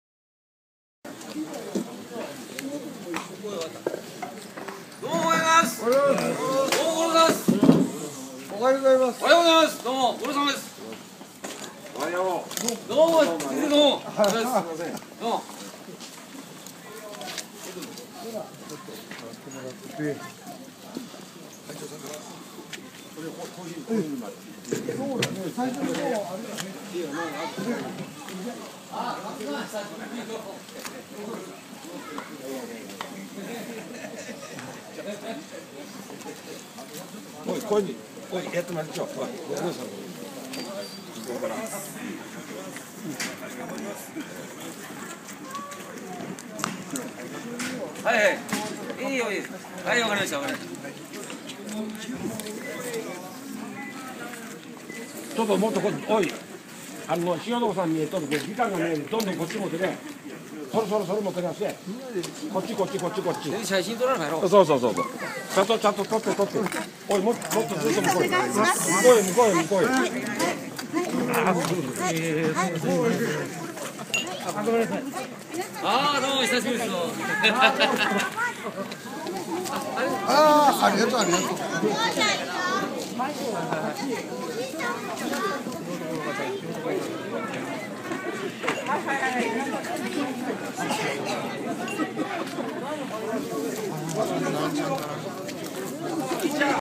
平成24年 国府宮はだか祭 神男「上田隆人」さん 松下奉賛会1
平成24年2月4日（土）に執り行われる愛知県稲沢市の国府宮はだか祭（儺追神事）。
1月29日（日）に松下奉賛会の餅つき安全祈願祭に、本年の神男となった上田隆人さんが鉄鉾会（てっしょうかい）の皆さんとともに本場より一足早く来訪されました。
（※画質を含め暫定UP版となります）